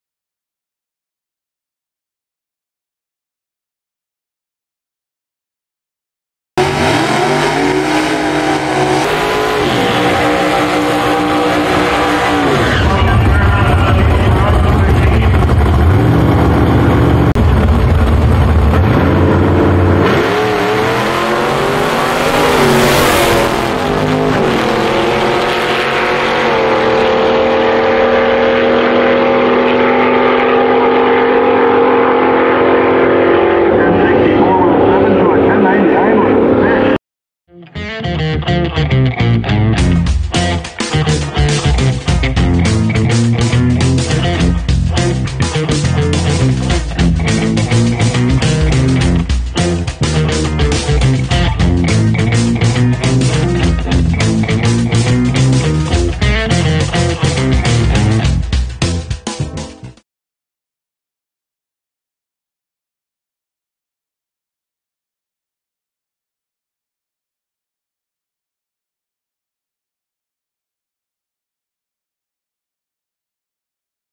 Brew City Gassers under the lights at the 2022 Out-A-Sight Drags #4
BCG Craig Ziegler in his 56 Chevy Wagon
vs
BCG Neal Brown in his 51 Chevy Gasser